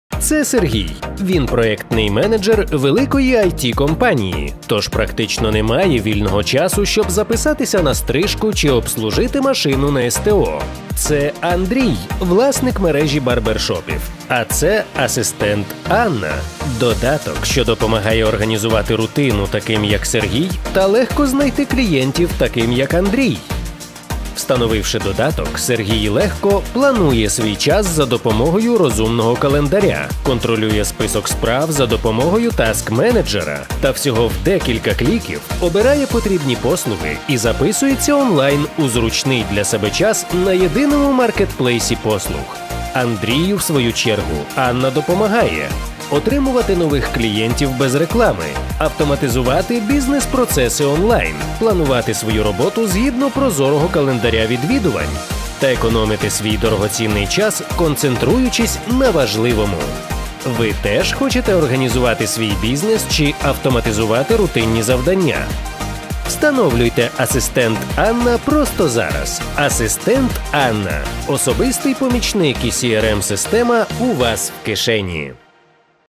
Скрінкаст | Анімаційне відео | Skreenkast | 2D | Асистент Анна | Студія Анімації 7030.studio
Переваги та відмінності вашого мобільного додатку найкраще може продемонструвати рекламне анімаційне відео з якскравою стилістикою та цікавими персонажами.
Нами був створений анімаційний ролик для мобільного додатку Асистент Анна, який привертає увагу з перших 5 секун.
Тож, звертайтеся за контактами, наведеними нижче і наша команда з радістю створить для вас рекламний ролик.
The best way to demonstrate the benefits and differences of your mobile app is with a promotional animated video with high-quality styling and interesting characters.
We created an animated commercial for the Assistant Anna mobile app that attracts attention from the first 5 seconds.
So, ask for the contacts below and our team will be happy to create a commercial for you."